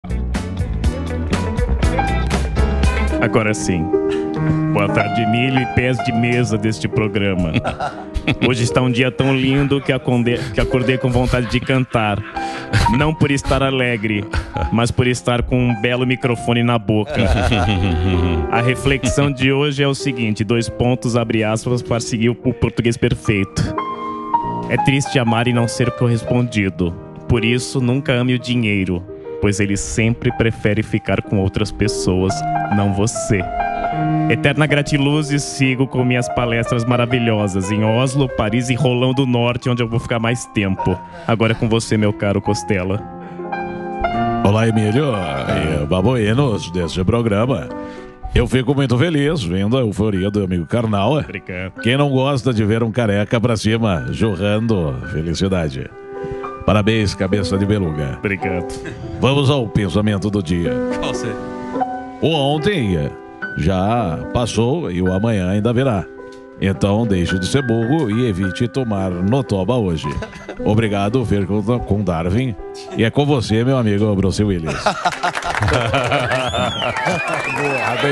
POTTER KARNAL E SERVE COSTELLA EM UMA SAUDÁVEL RINHA DE PENSADORES
As reflexões mais absurdas da internê brasileira com Leandro Potter Karnal, o careca mais feliz da região central de SP, e Mário Serve Costella, o gordinho do pastel e pensamentos diários.